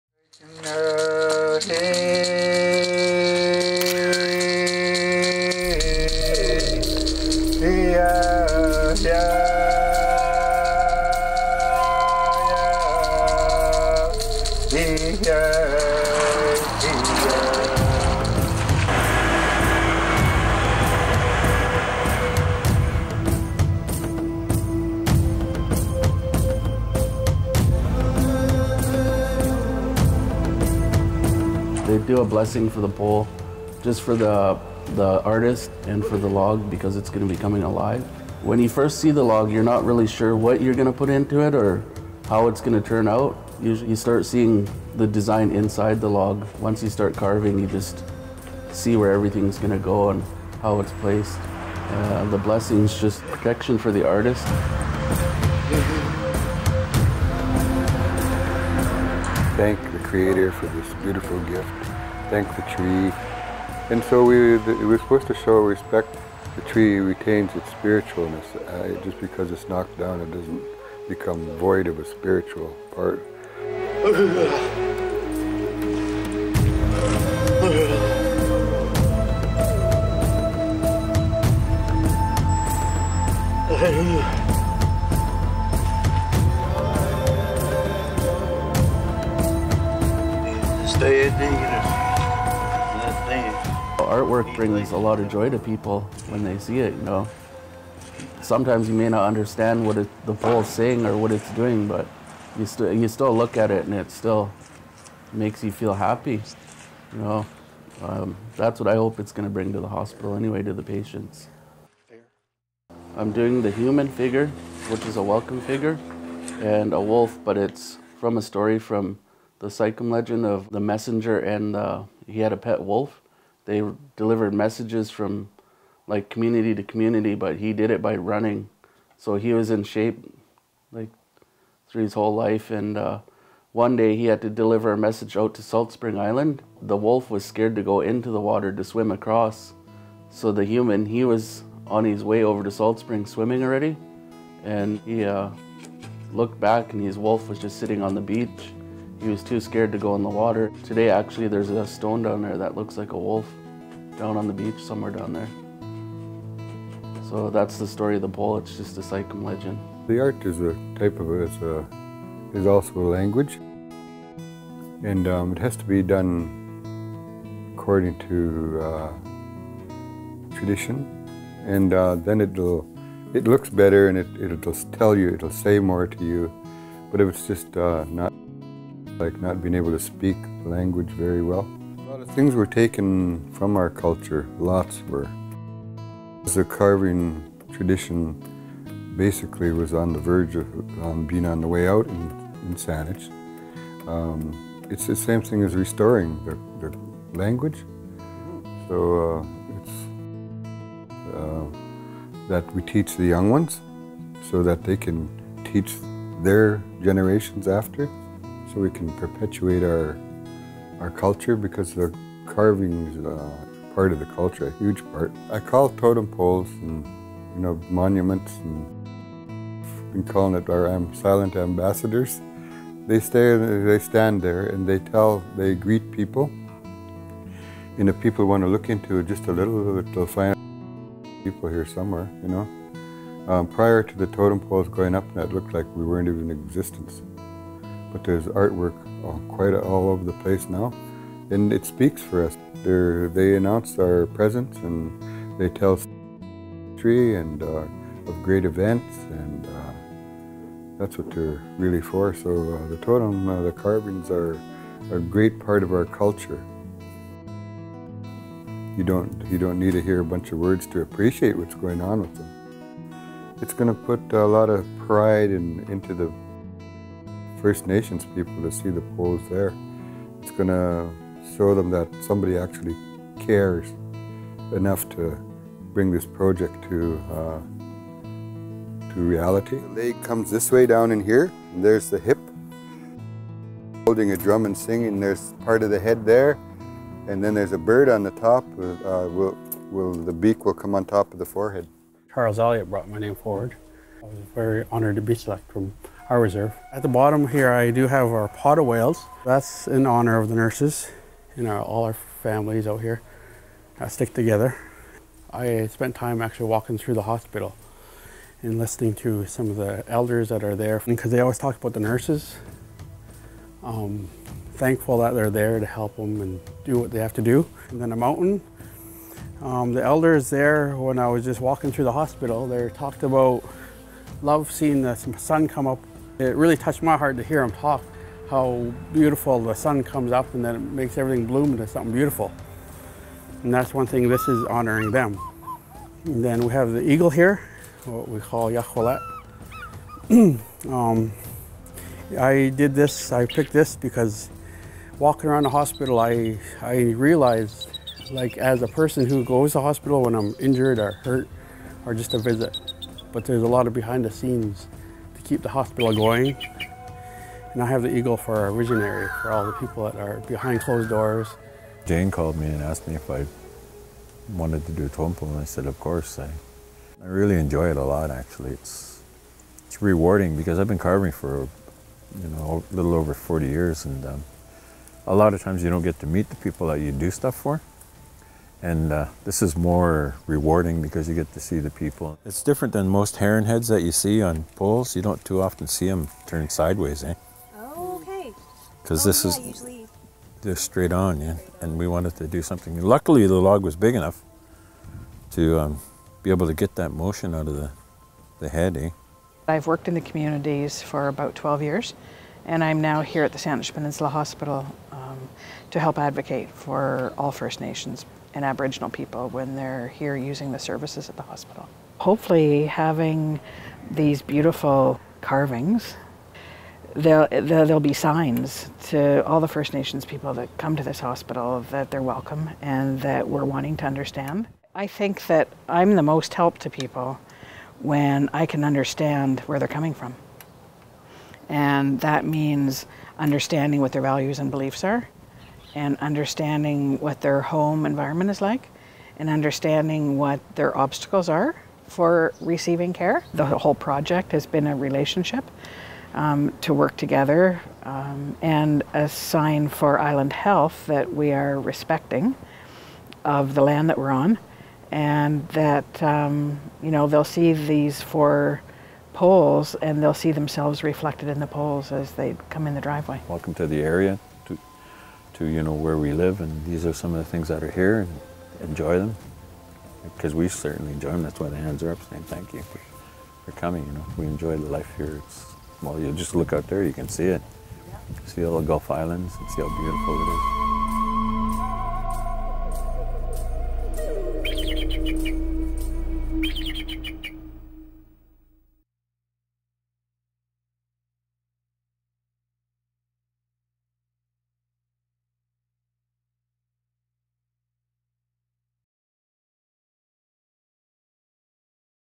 Out of the Wood - Totem Poles Celebrate Connection of Culture and Care at Saanich Peninsula Hospital
A collaboration driven by First Nations communities on Vancouver Island supported by a number of partnerships has resulted in four 15-foot, locally carved cedar totem poles raised at the Saanich Peninsula Hospital on the traditional territory of the W̱SÁNEĆ First Nation communities.